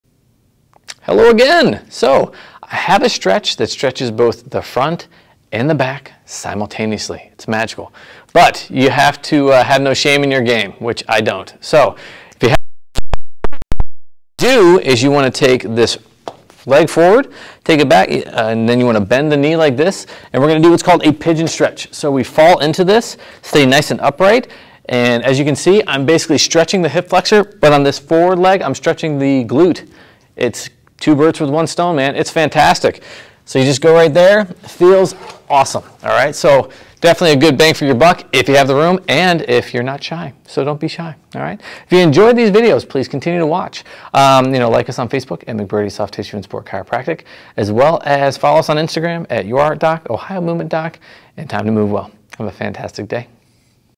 The Double WHAMMY BOOTY and HIP stretch! | McBrearty Chiropractic | Best Chiropractor Canton Ohio
In this video we demonstrate a stretch that effectively stretches the hip and booty on one side of your body while effectively stretching the Psoas/hip flexor on the other side of the body.  This is a very economical method for stretching out the hips when you are short on time.

If you are looking for a Chiropractor Canton Ohio, McBrearty Soft Tissue and Sport Chiropractic is taking new patients!